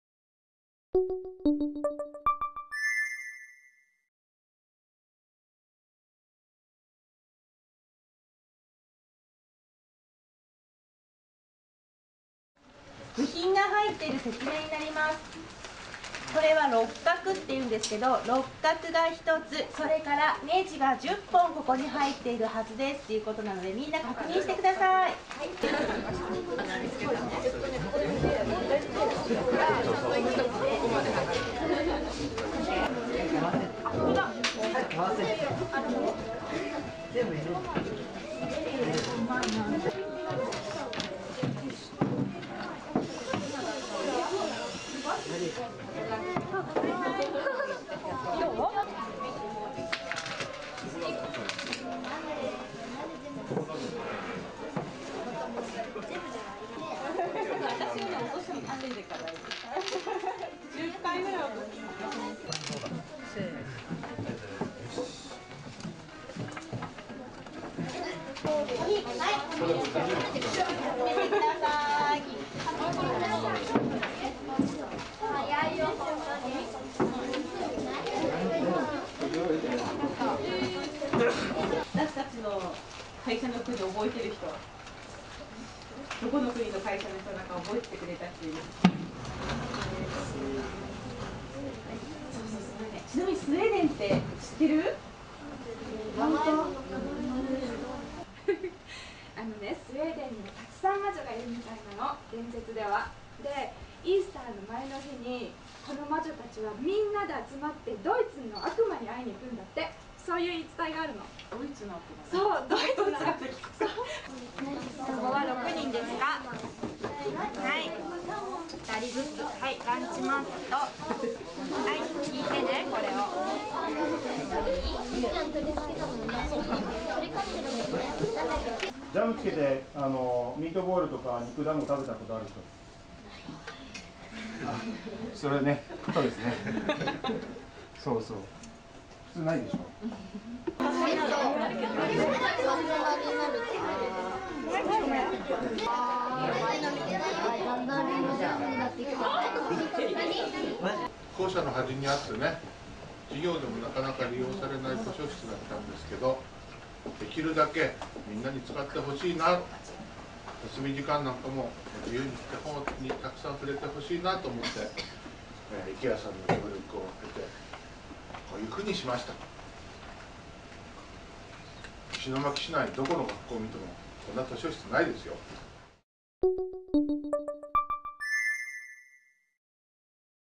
IKEAが小学校にやってきた
石巻市立貞山小学校に家具支援したIKEAですが、一方的な支援だけではなく、生徒たちにも作業してもらいました。作業を体験してもらい、そして、その家具が図書室に置かれるとなれば、図書室へ一層の愛着が湧くでしょう。今回与えられた組立家具は椅子です。そして、それが完成したら、予め用意していた色とりどりのカバーを被せて完成！

更に、組立作業の後にhIKEA発祥の地であるスウェーデンの文化の一端に触れる取り組みも行いました。スウェーデンの場所や気候、言い伝え、食文化など、小学生ではまだ習っていないであろう話を、面白おかしく教えています。生徒たちは興味津々で聞き入ります。

そして、お待ちかね。試食の時間です。IKEAフードのスタッフの方が作られたスウェーデンの典型的な家庭料理が振舞われます。石巻でも馴染みがある物を、ということでサーモンも使われていました。その他にはフルーツソースとグレイビーで食べるミートボールなど、日本の食文化には無いような食事も供されます。私も勧められるままに食べましたが、最高に美味しかったです。今度、皆様もミートボールにジャムを付けて食べてみませんか？新たな発見が有るやも知れません。

新しい図書室を得ただけではなく、スウェーデンの文化にも触れた生徒達、今日のことを忘れないでしょう。今回異文化を知ったことが彼らの将来に何か役立つことを願ってやみません。

IKEA　東日本こどもプロジェクト